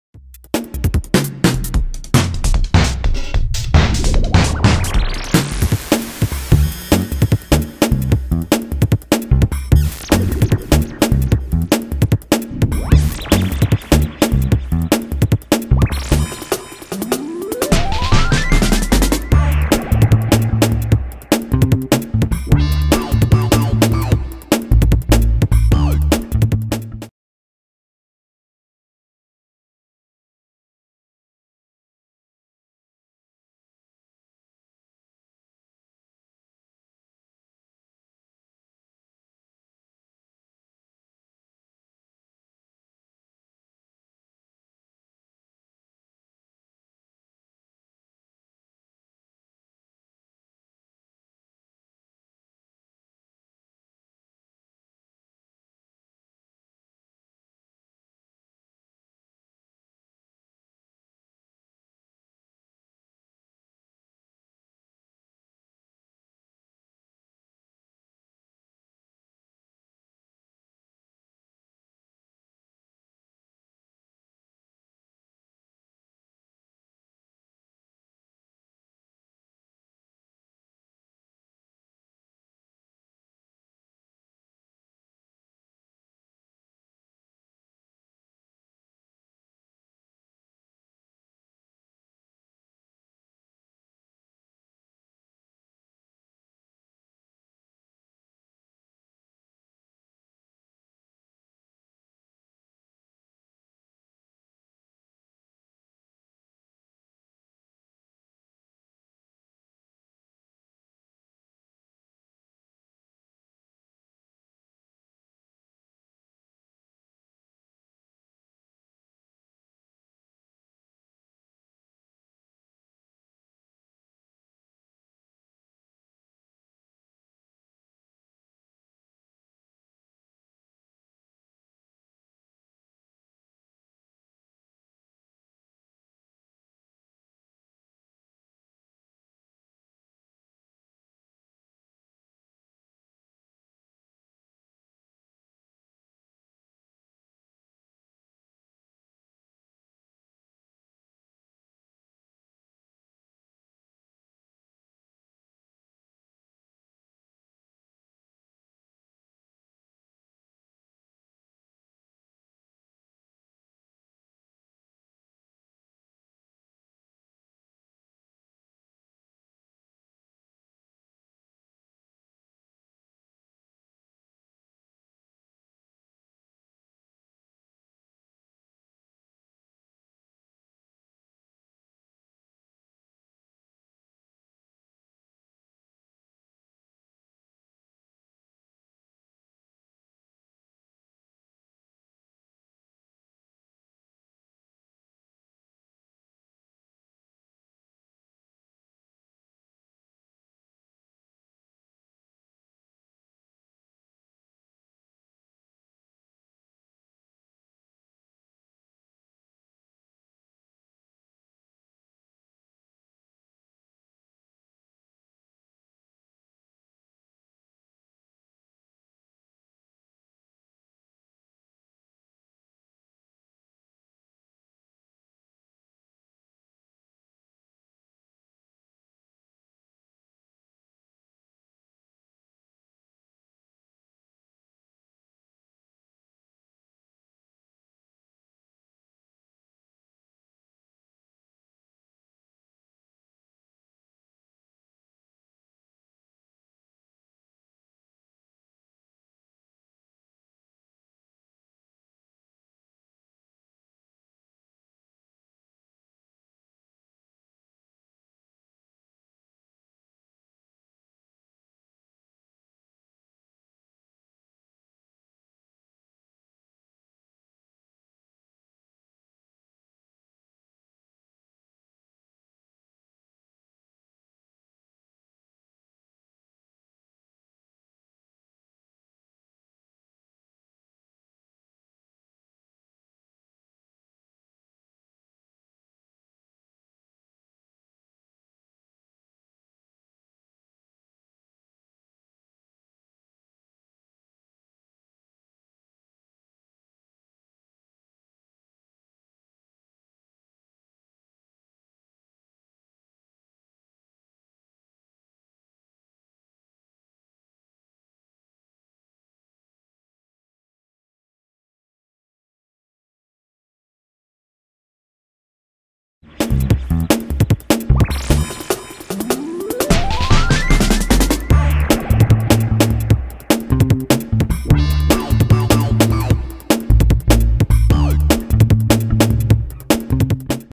Tinkercad Tutorial - Fish 3D
This a graphical representation of a 3d model of a fish.